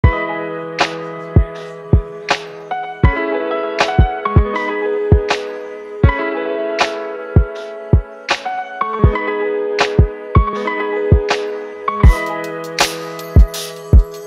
Using terracotta pigments to make home decor
See a little behind the scenes into a handmade rainbow tray with jesmonite AC100. Home decor for handmade products. All handmade in our studio in the Netherlands.